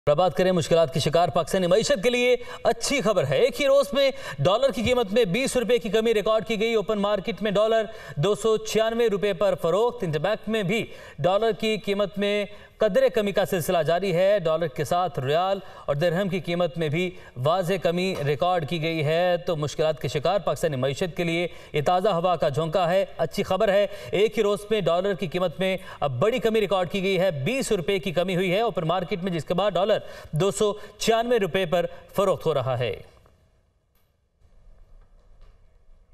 Minister of Finance Ishaq Dar Gives Surprise To Dollar | Dunya News
Rupee gains massively after new credit card settlement directive